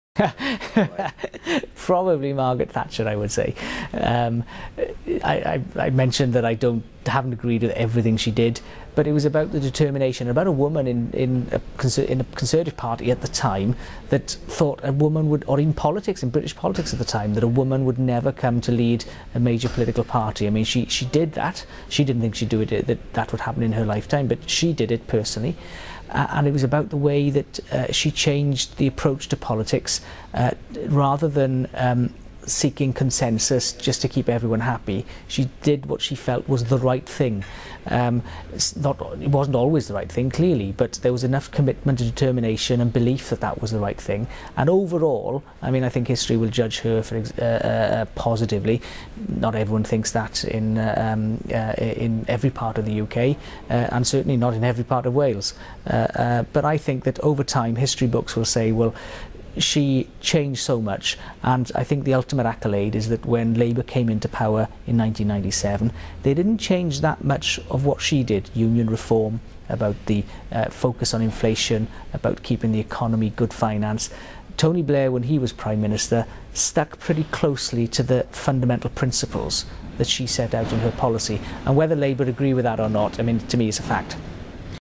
Alun Cairns Interview Q:8
Who is your political hero and why?